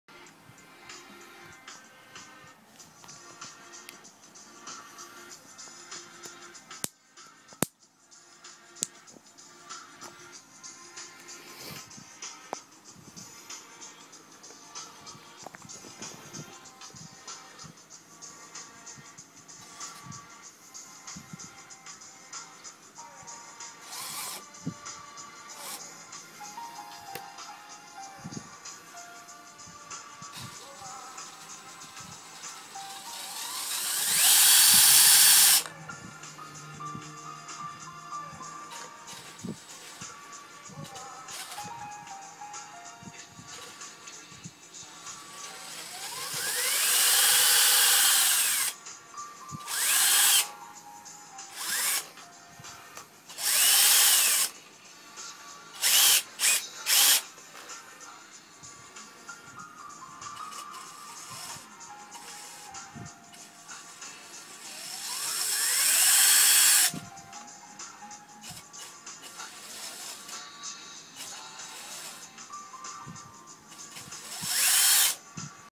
Banebots brushless esc
Test of banebots wired to a turnigy 85a brushless esc... Slow speed control example